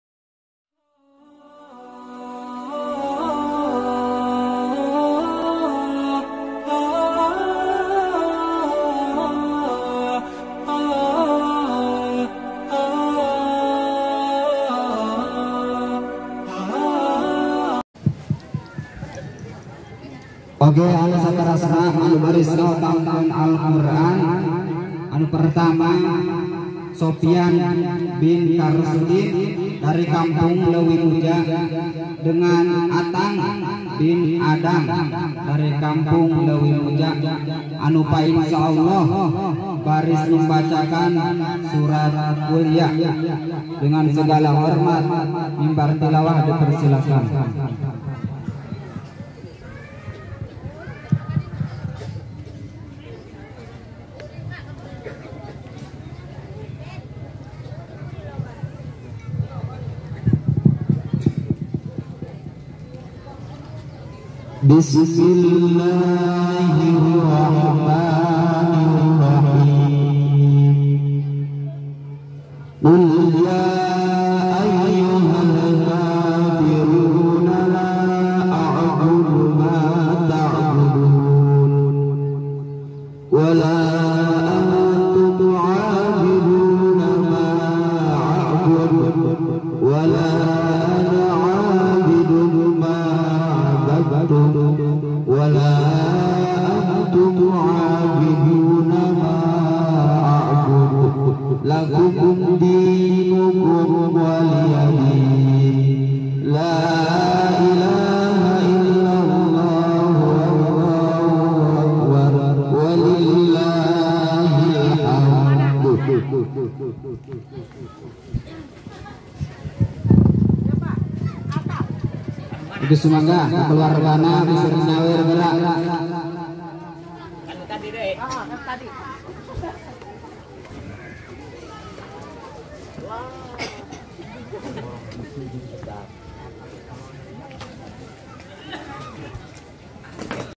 Lantunan Ayat Suci Al-Qur'an Yang Ke Delapan Di Bawakan Oleh Ustad Sopian Dan Ustad Atang
Asalamualaikum wr.wb selamat datang di chanel rab sbr  acara khotmil qur'an para santri Putra putri Ponpes Riyadul Awamil Kampung Barokah citarum Desa Cikiruhwetan Kecamatan Cikeusik Kabupaten Pandeglang Banten Pada Hari Selasa Tgl 1 Maret 2022